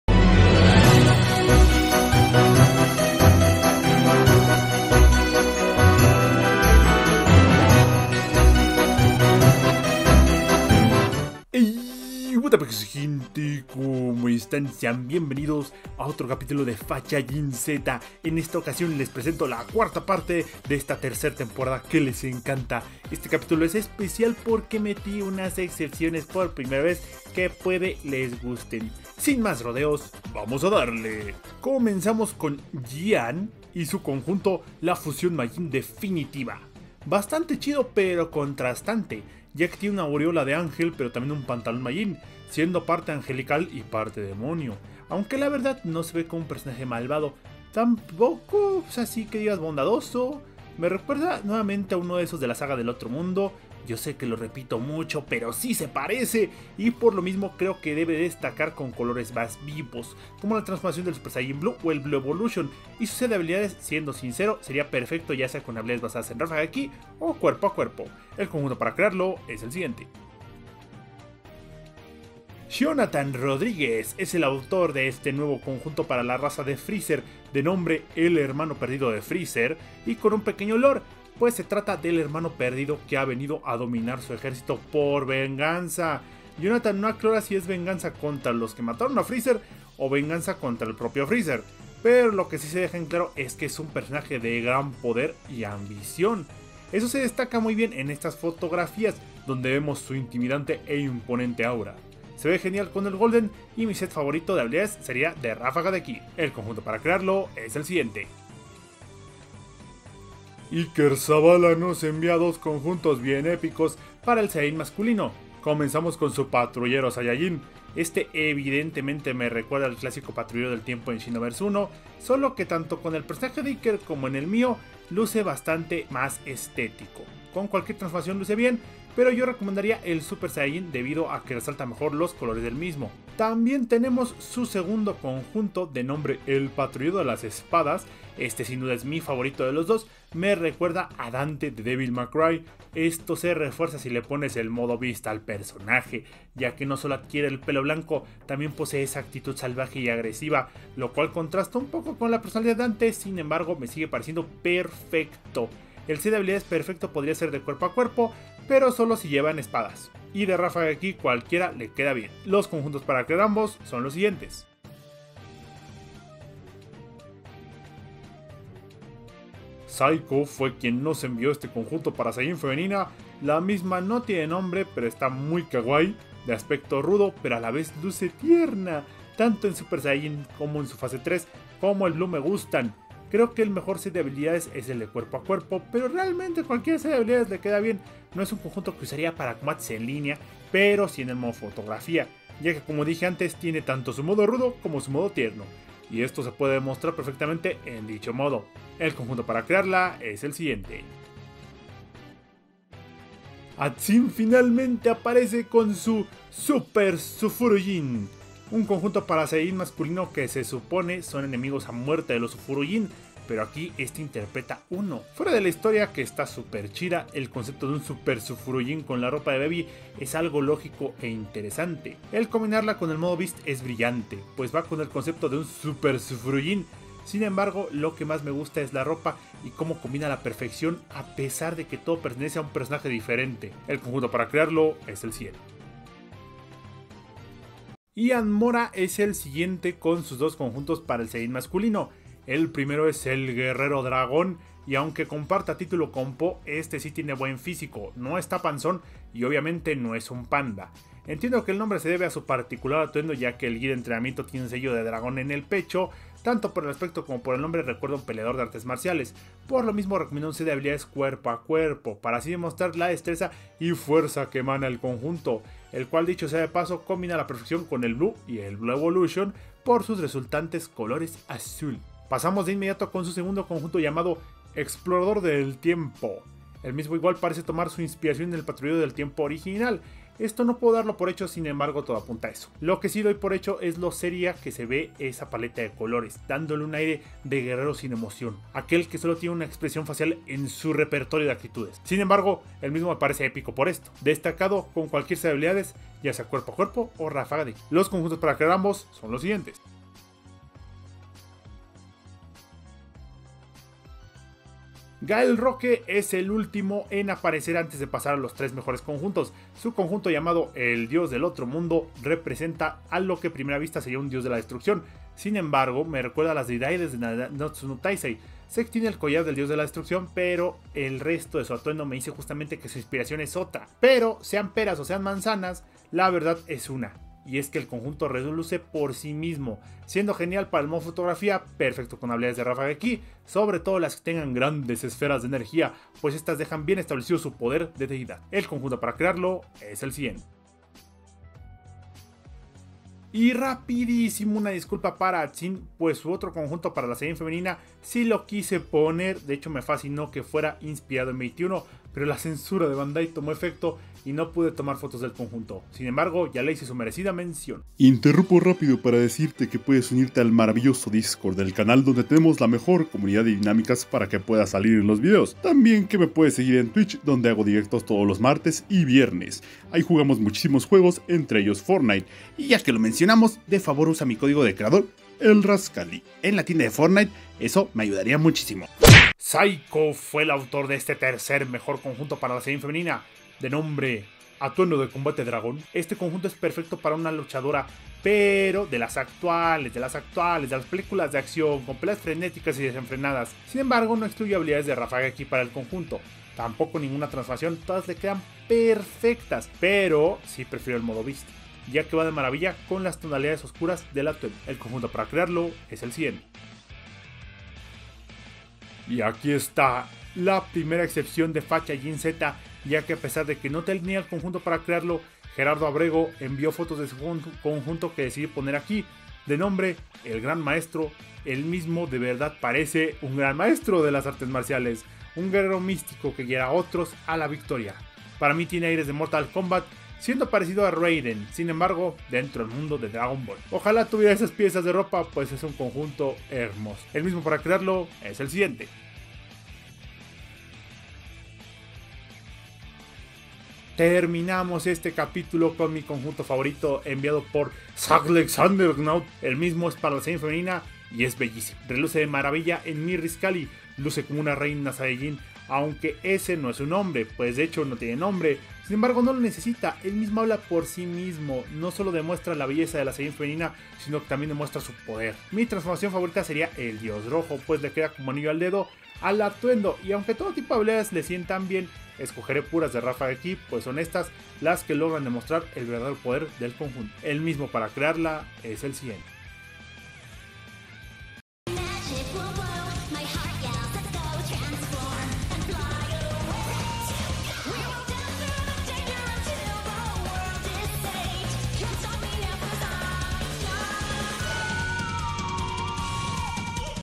LOS MEJORES CONJUNTOS DE SUSCRIPTORES | Fachajin Z | S3 E4
Ey what a peks gente ¿Cómo están? Sean bienvenidos a otro capítulo de FACHAJIN Z, el día de hoy con la cuarta entrega de esta tercer temporada, como verán salió en miércoles debido a que estuve enfermo, pero ya estoy sano y hemos vuelto al ritmo habitual, espero que lo disfruten  :D